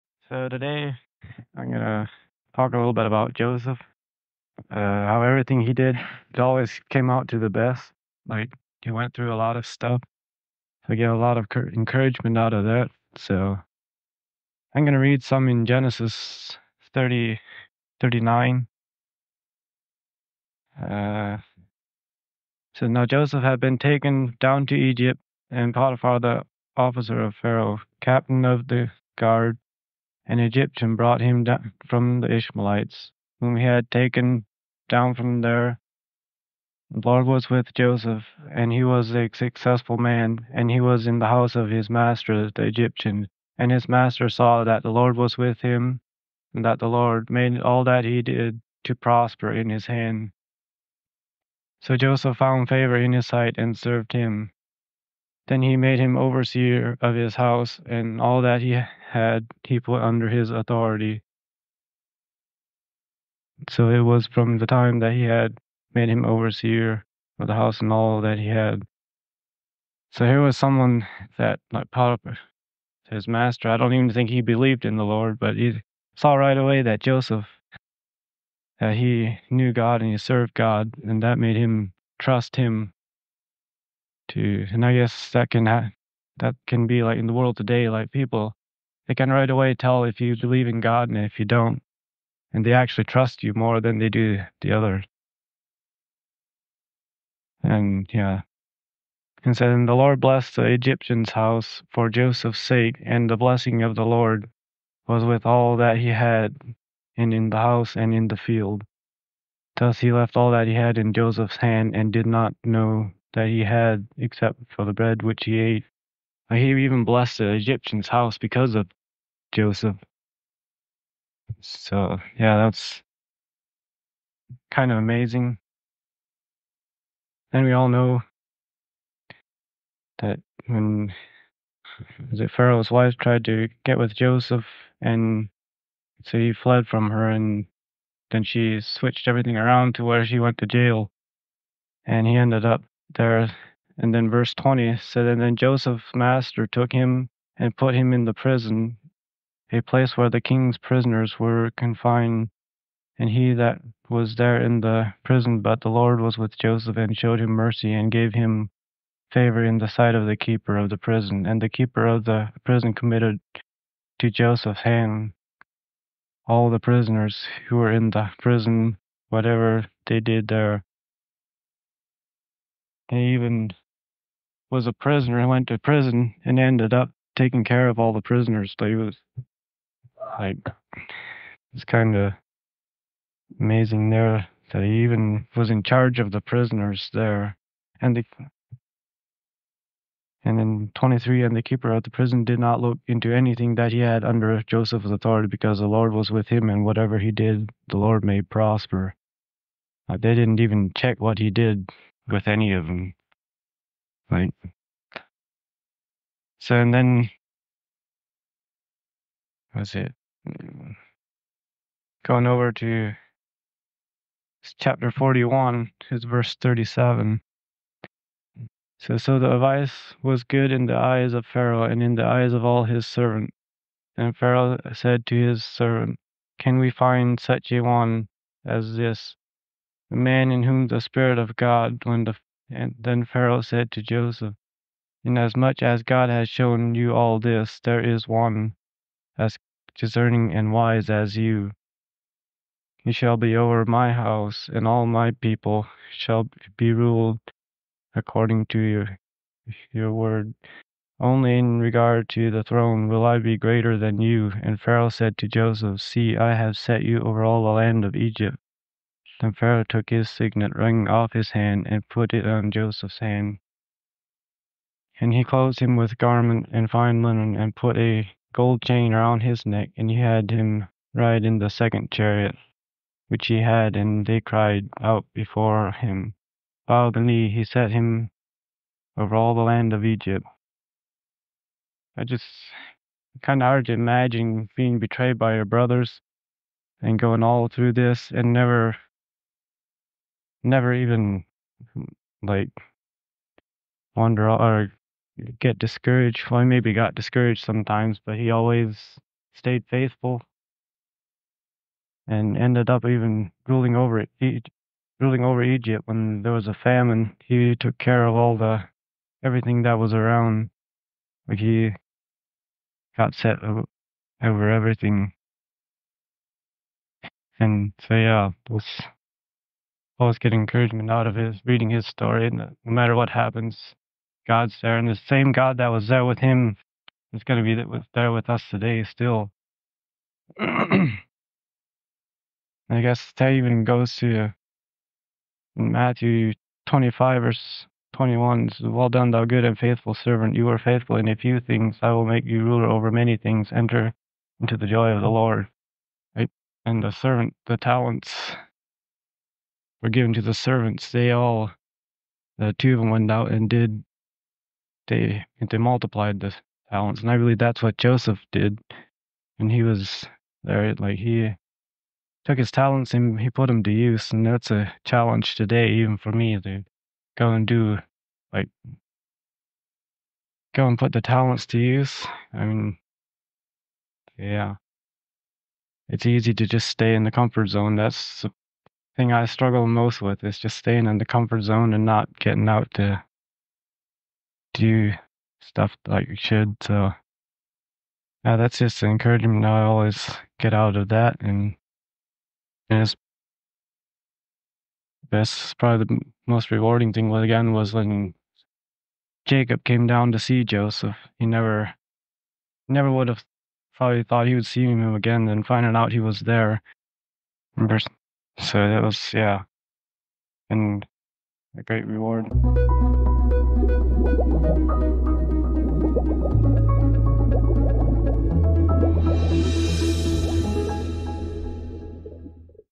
Joseph: The Blessing of Faithfulness
Allegheny Mission Church message from November 3, 2024 by Wayne Mullet